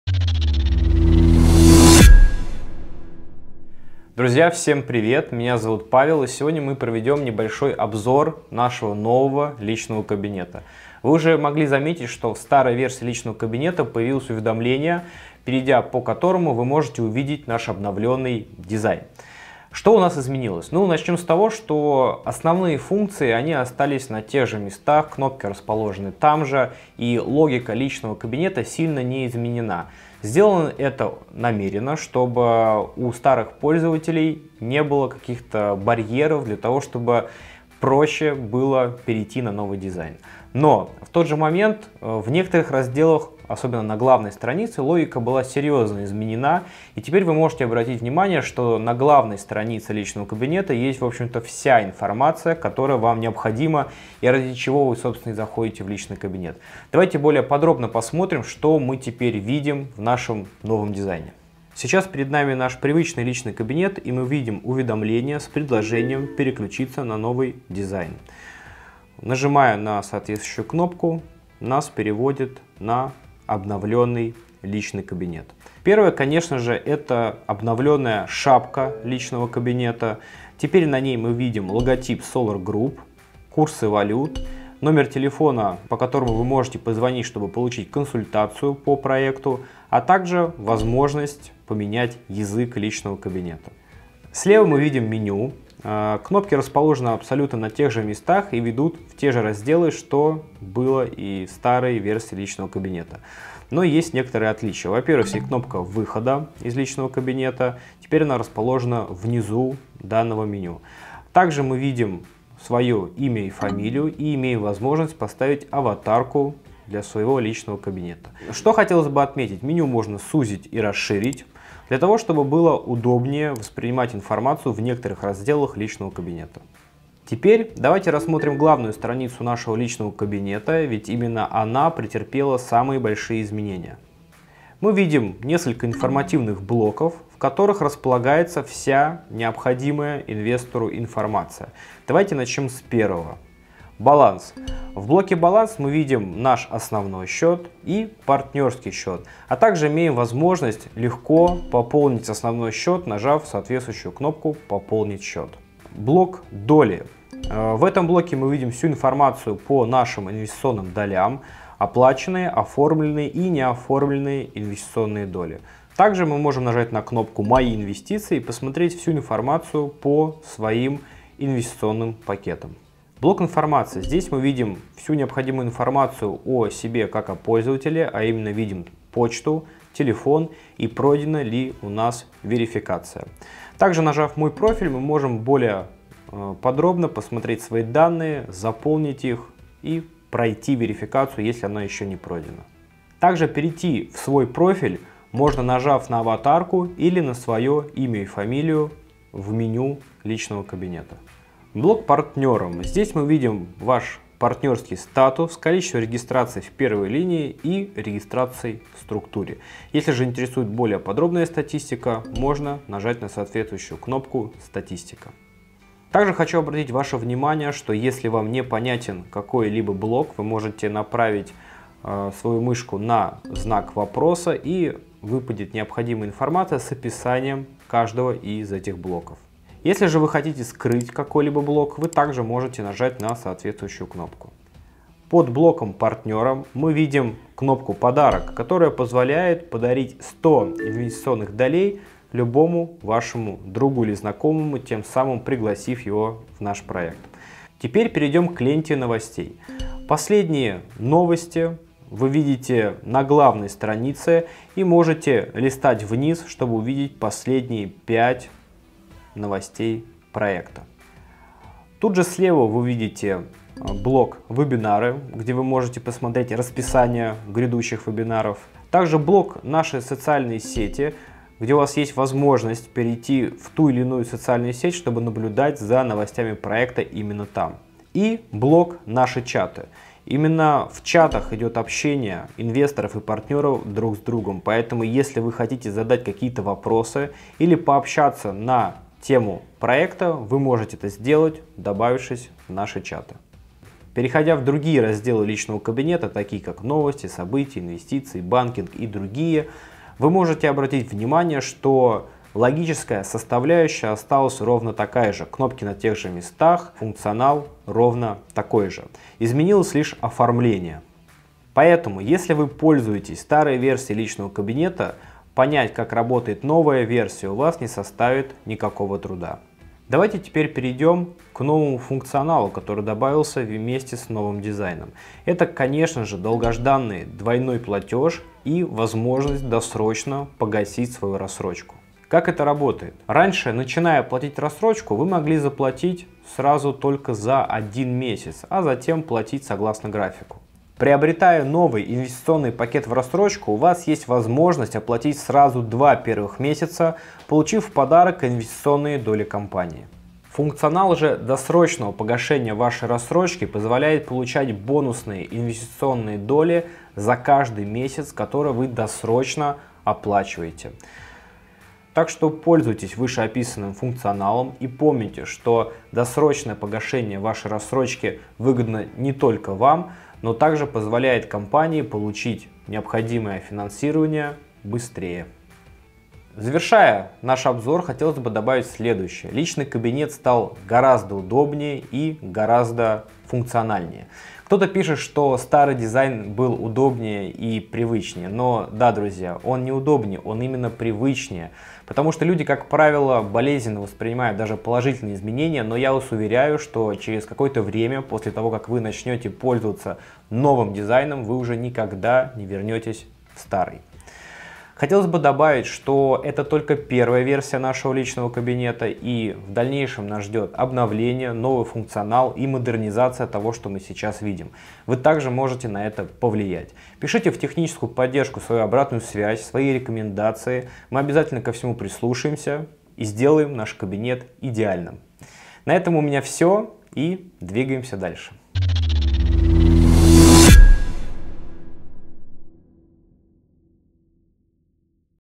Видеообзор обновленного личного кабинета l Проект "Двигатели Дуюнова" сайт - solargroup (П.Филиппов)
Для тех, кто ещё не успел познакомиться с обновленным дизайном личного кабинета, мы записали подробную видеоинструкцию.

Павел Филиппов рассказывает о том, как изменился внешний облик и функционал личного кабинета. Показывает, какие появились возможности у инвесторов и партнеров. В том числе демонстрирует, как воспользоваться новыми опциями "двойной платеж" и "досрочное погашение".

Осваивайте функционал личного кабинета и вносите свои рациональные предложения по его улучшению в техническую поддержку! Мы продолжим совершенствовать личный кабинет и будем рады обратной связи от Вас.